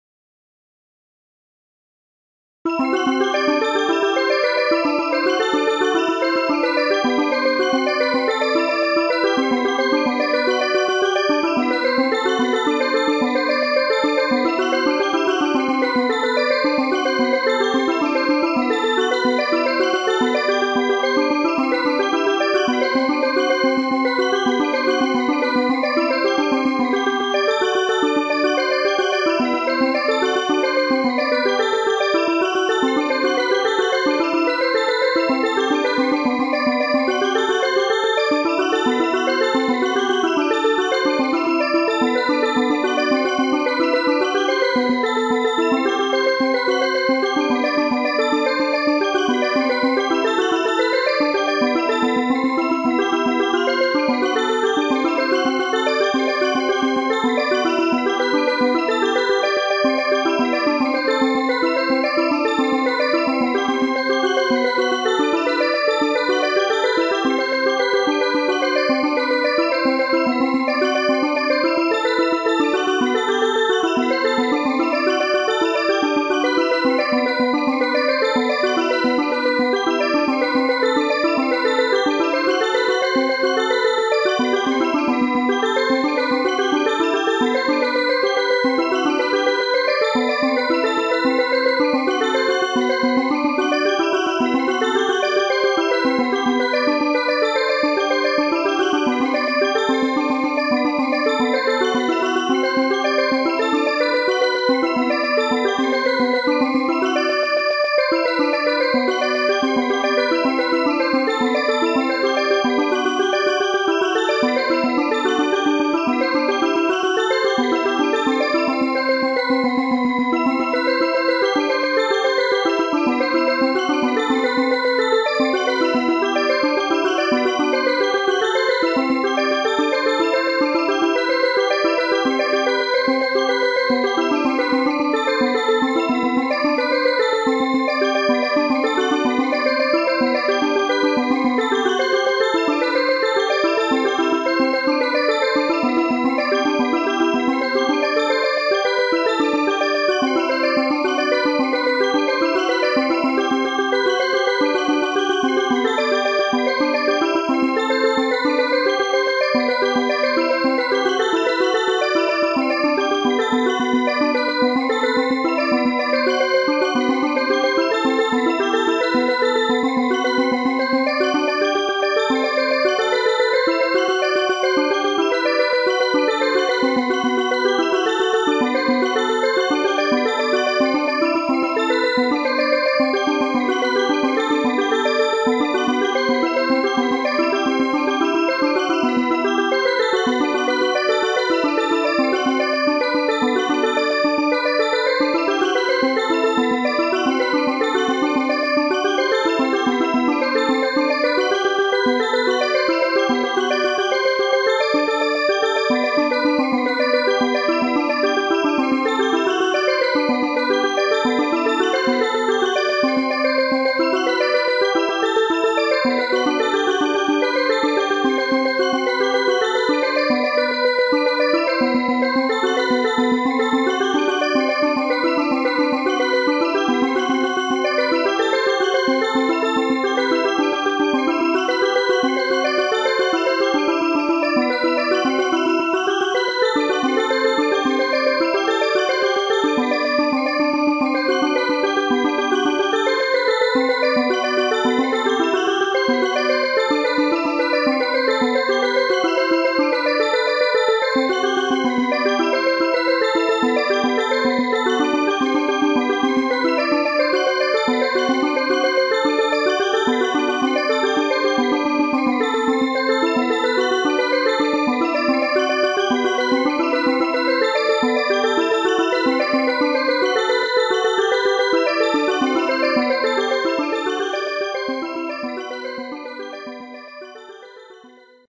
円周率2009 ト長調 Pi ４分３０秒版
ほかの動画にインスパイアされて作りました。
0~9をシドレミファ＃ソラシドレに対応させてます
全部で２００９ケタ

テンポ175です。